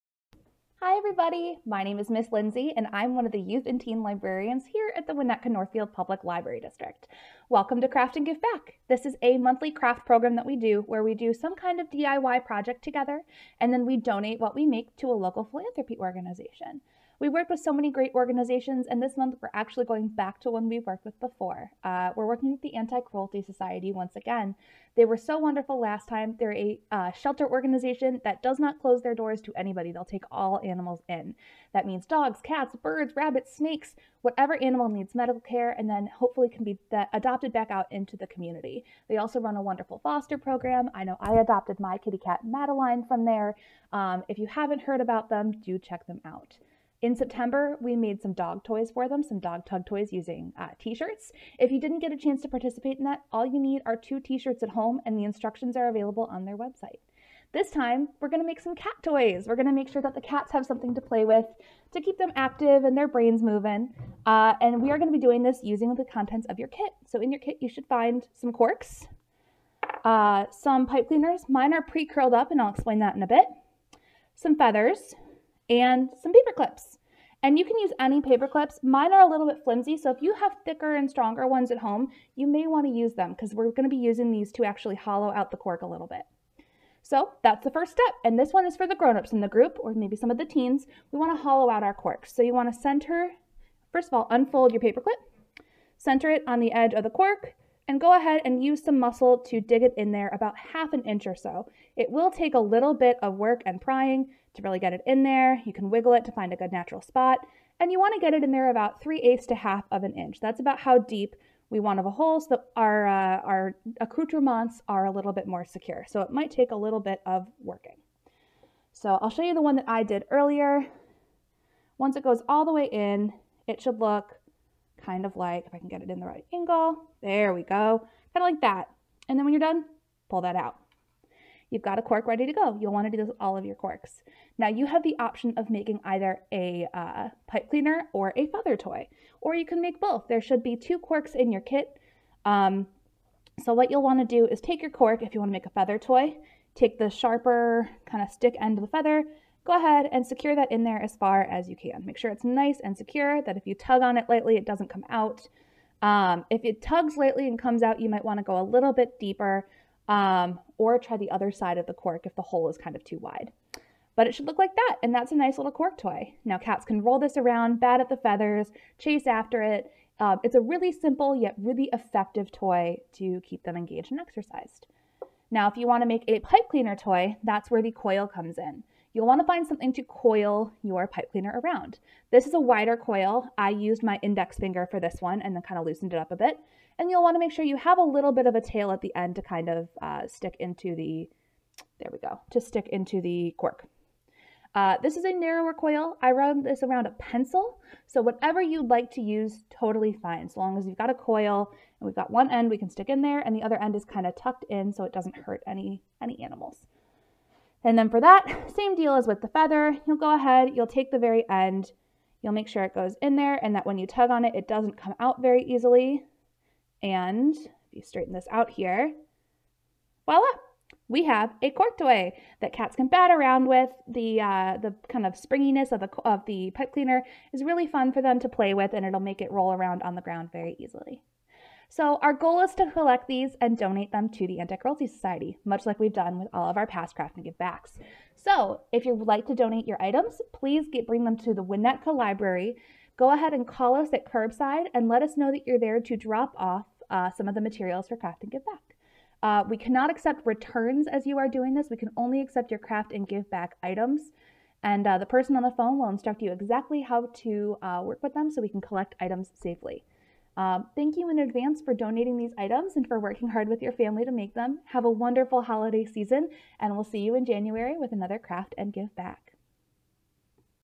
Craft and Give Back - Cork Cat Toys
Make a fun and easy cork cat toy with Miss Lindsay! This month, we're donating our DIY projects to Anti-Cruelty Society Chicago.